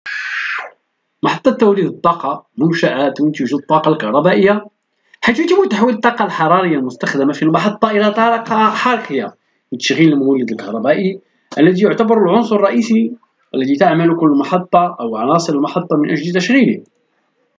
محطة توليد طاقة كهربائية
محطة توليد الطاقة منشأة تُنتج الطاقة الكهربائية حيث يتم تحويل الطاقة الحرارية المستخدمة في المحطة إلى طاقة حركة لتشغيل المولد الكهربي الذي يعتبر العنصر الرئيس الذي تعمل كل عناصرالمحطة من أجل تشغيله.

محتويات
1 أنواع محطات الطاقة
2 المحطات الحرارية
2.1 تصنيف المحطات الحرارية
3 محطات الطاقة المائية
4 محطات الطاقة الشمسية
5 مزارع الرياح
6 اقرأ أيضا
7 مراجع
أنواع محطات الطاقة
محطات طاقة حرارية
محطات طاقة مائية
محطات طاقة شمسية
محطات طاقة الرياح
محطات طاقة امواج البحر
محطات نووية
محطات هواء المضغوط
محطات غازية
محطات هيدروليكية
محطات ديزل
المحطات الحرارية
Crystal Clear app kdict.png مقالة مفصلة: محطة حرارية
و هي التي يتم فيها تحويل الطاقة الحرارية -والتي غالبا ما تنتج عن احتراق وقود- إلى طاقة حركية لتوليد الكهرباء. وهذا النوع من المحطات هو الغالب على مستوى العالم.

تصنيف المحطات الحرارية

إحدى محطات توليد الطاقة الكهربائية باستخدام الطاقة الحرارية الأرضية في أيسلاندا
محطات بخارية : تستخدم التوربين البخاري
محطات احتراق داخلي :تستخدم التوربين الغازي
محطات مفاعلات نووية: تستخدم الوقود النووي يورانيوم
محطات الحرارة الأرضية: تستخدم الطاقة الحرارية الأرضية
محطات المخلفات: تستخدم المخلفات الصناعية كوقود.
محطات الطاقة المائية
Crystal Clear app kdict.png مقالة مفصلة: طاقة كهرمائية
و يتم فيها تحويل طاقة الوضع التي تختزنها المياه في المناطق المرتفعة كأعالي السدود والشلالات إلى طاقة حركية لتدير توربين مائي والذي بدوره يدير المولد الكهربي.

محطات الطاقة الشمسية
Crystal Clear app kdict.png مقالة مفصلة: طاقة شمسية

محطة طاقة شمسية في البرتغال
تستخدم فيها الخلايا الشمسية لتحويل أشعة الشمس مباشرة إلى كهرباء، وقد تحتاج هذه الطريقة إلى تحويل التيار إلى تيار متردد لنقله إلى المستهلك، ولكنها تتميز بعدم وجود أجزاء ميكانيكية وسيطة لتحويل الطاقة.

هناك أنواع أخرى لمحطات الطاقة الشمسية كتجميع أشعة الشمس لتسخين الماء ثم استغلال نفس فكرة المحطات الحرارية لتدوير المولد الكهربائي. وهناك محطات البرج المركزي التي تستخدم المرايا لتجميع أشعة الشمس وتسخين الهواء والذي بدوره يرتفع إلى أعلى في برج يحتوي على المولدات الكهربية التي تعتمد على هذا الهواء الساخن في الدوران كما تستطيع حفظ هذه الحرارة في مادة كالصوديوم لاستغلالها في توليد الطاقة حال غياب الشمس.

مزارع الرياح
Crystal Clear app kdict.png مقالة مفصلة: طاقة الرياح
Eolienne et centrale thermique Nuon Sloterdijk.jpg
يمكن استخدام طاقة الرياح في المناطق التي تتمتع برياح سريعة وقوية لتدوير طواحين الهواء والتي بدورها تستخدم في توليد الطاقة الكهربائية. يميز هذه الطاقة أنها نظيفة تماما ولا ينتج عنها أي تلوث ولكن يعيبها أنها بحاجة إلى مساحات واسعة وأن أماكن استغلال طاقة الرياح محدودة وبعيدة. وعلاوة غلى ذلك فثمن الكيلوواط منها يمثل ضعف الكيلوواط المنتج بالفحم أو النفط على الأقل. ولهذا يتطلب هذا النوع من إنتاج الطاقة إلى دعم كبير حكومي كما في ألمانيا.

اقرأ أيضا